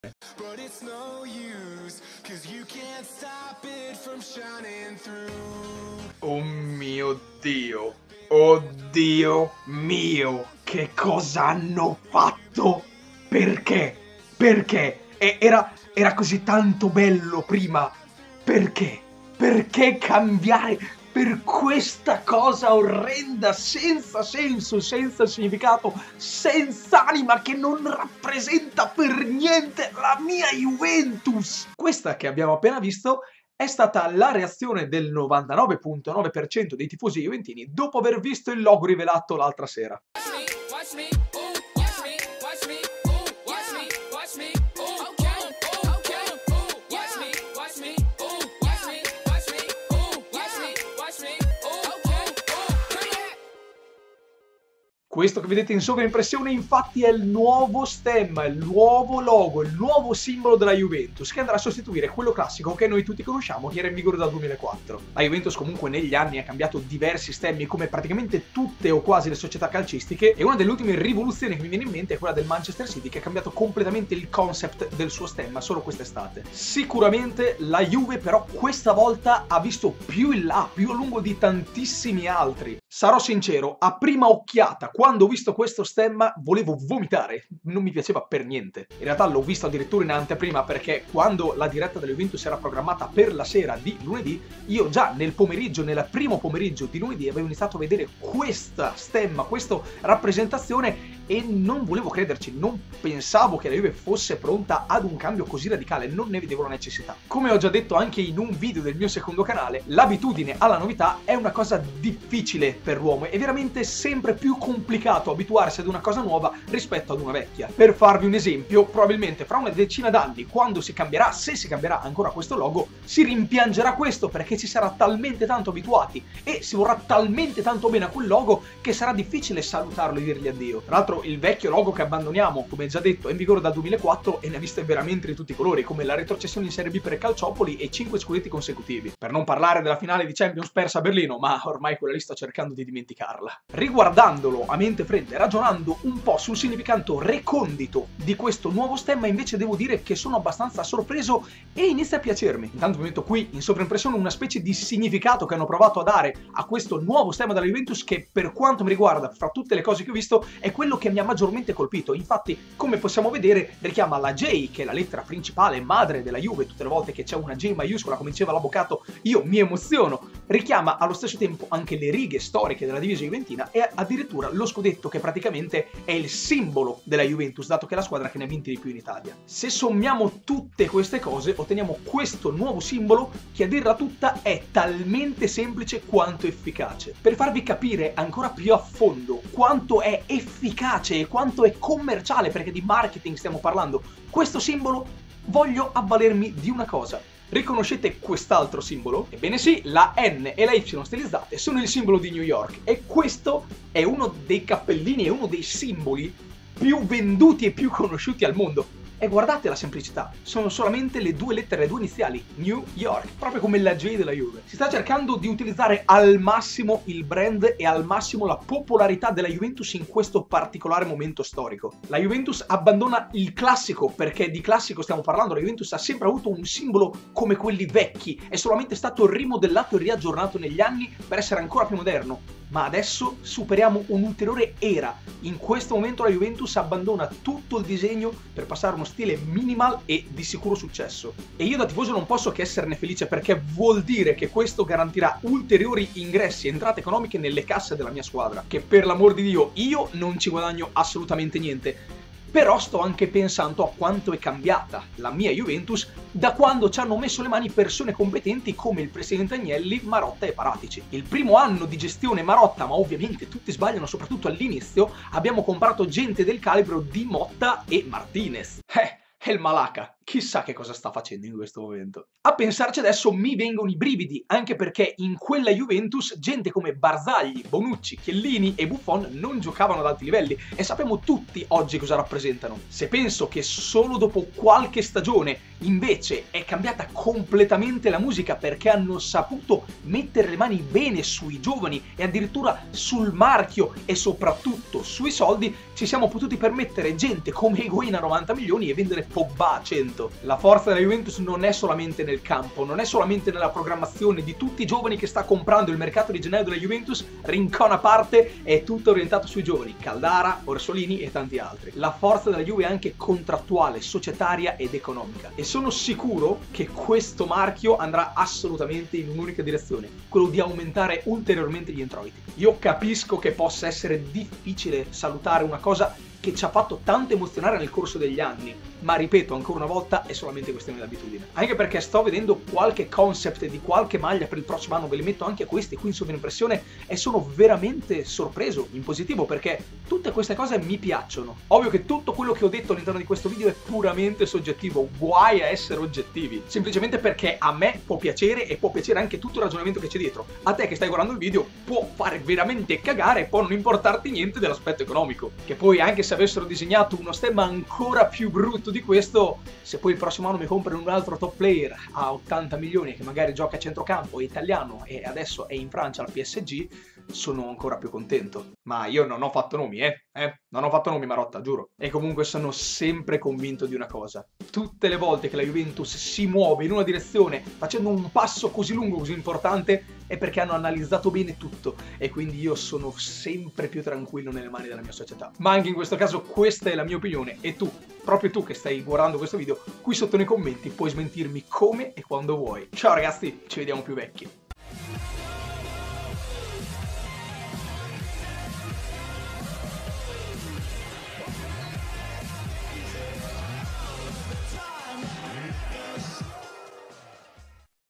JUVENTUS: NUOVO LOGO E MAGLIE! LA REAZIONE DI UN TIFOSO JUVENTINO!
SEGUI I MIEI SOCIAL